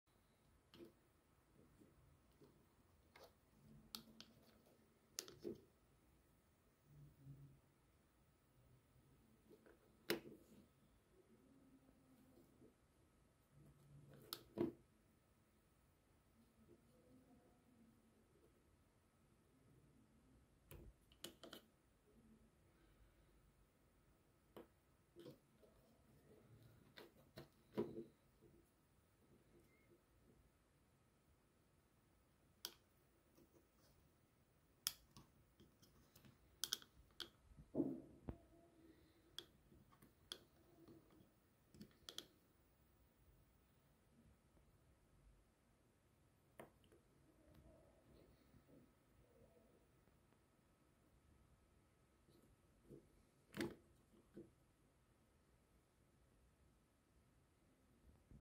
FIX IT! Right/Left Earbud Basic 2 not working
Here, I show you how to fix the right or left bud (Xiaomi Mi Earbuds Basic 2) if it suddenly stopped working and doesn't turn on.

All you have to do is to put it back on the case while pressing the button. You could also try to press it right after it touches the case.

If it doesn't work, keep trying. It will :)

I hope this works. It happened to me before with the left earbud. It seems to be really common so don't stress out.